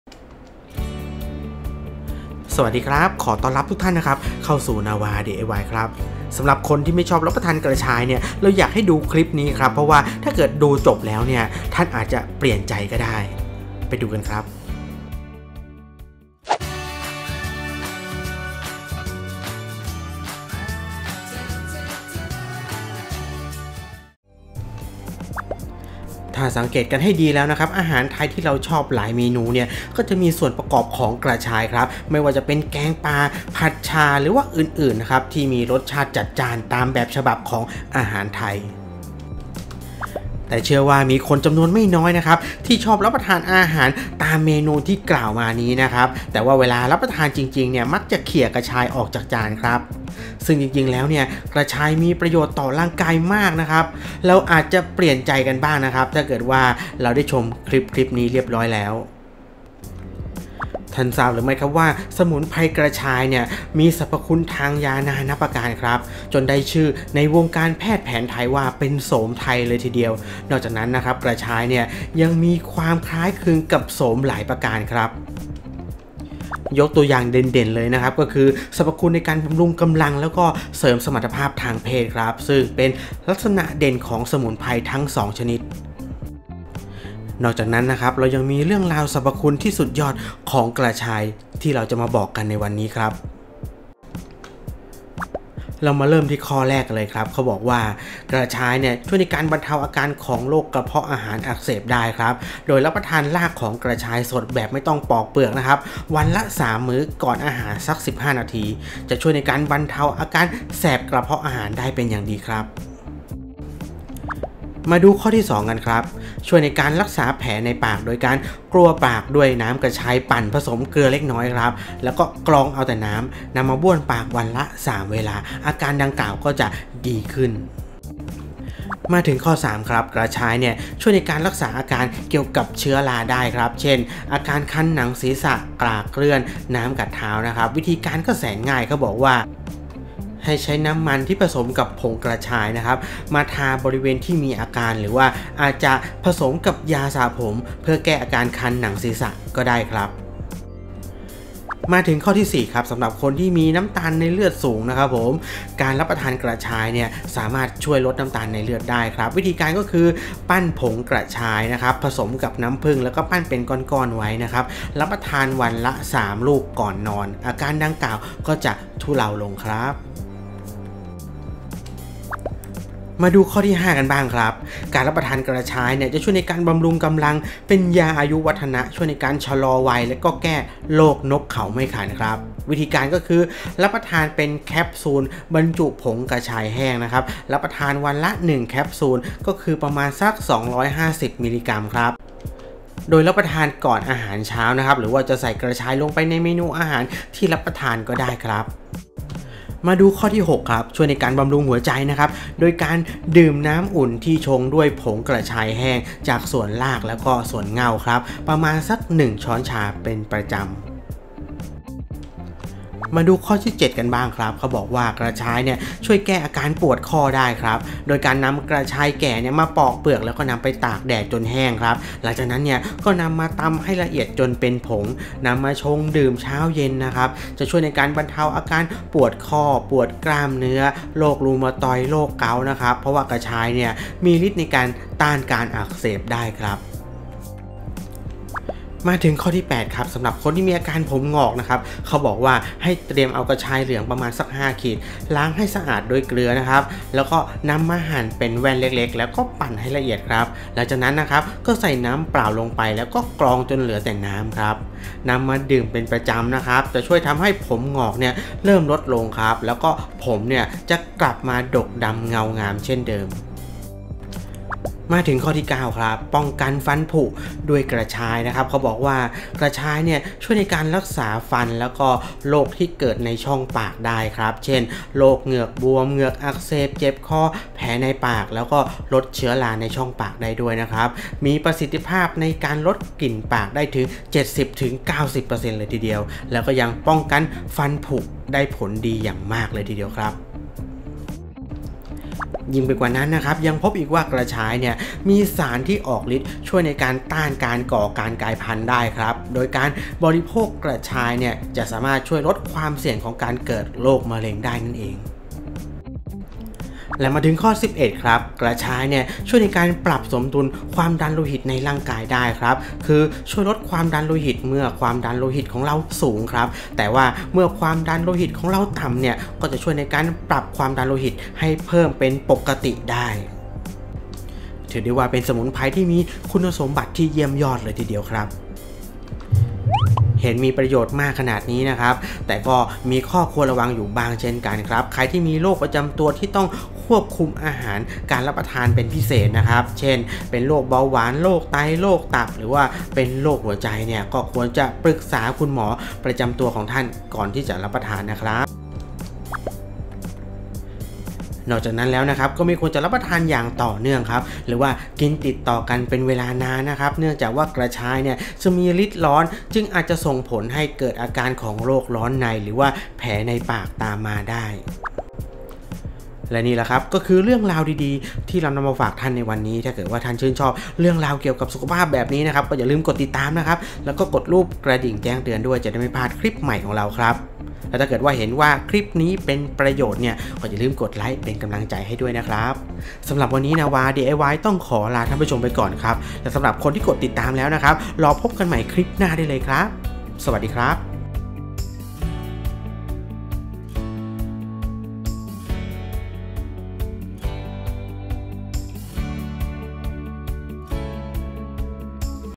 ตาสว่าง..!! "กระชาย" 11 โรค แทบไม่ต้องหาหมอ แต่หลายคนยังไม่รู้ | Nava DIY
ตาสว่าง..!! 11 โรค แทบไม่ต้องหาหมอ "กระชาย"  แต่หลายคนยังไม่รู้